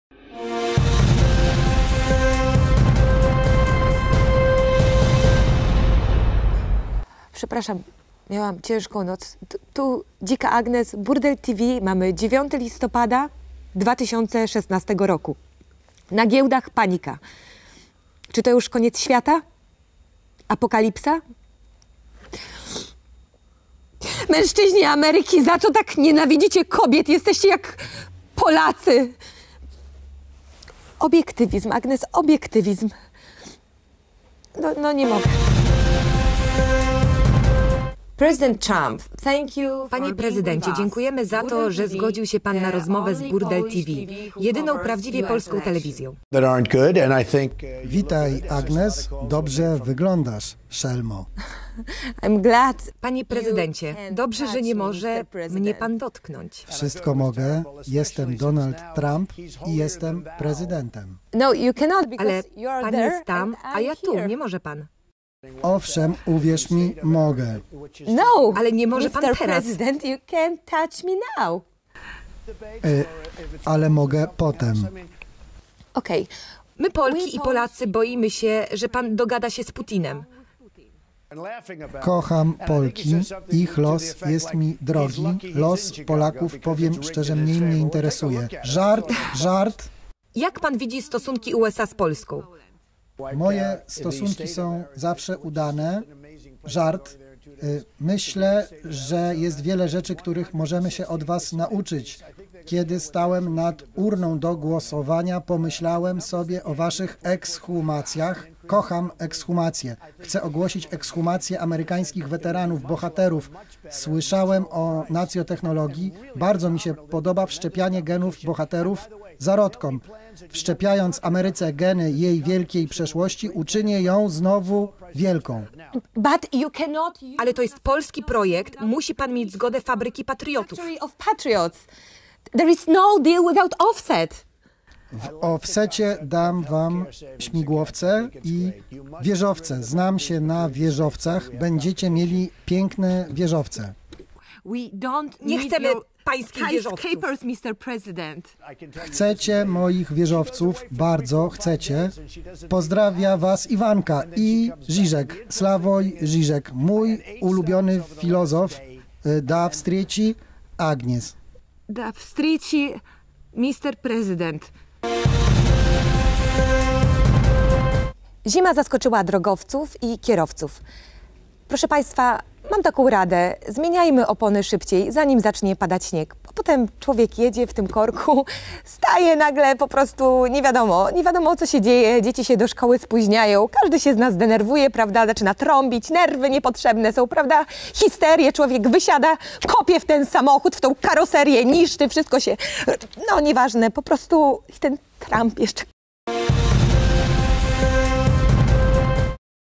BurdelTV 11/9 Wywiad z Prezydentem Elektem Donaldem Trumpem
Pierwszy wywiad z 45 Prezydentem Elektem Stanów Zjednoczonych Ameryki Północnej Donaldem Johnem Trumpem w polskiej telewizji. BurdelTV jak zawsze najszybsza.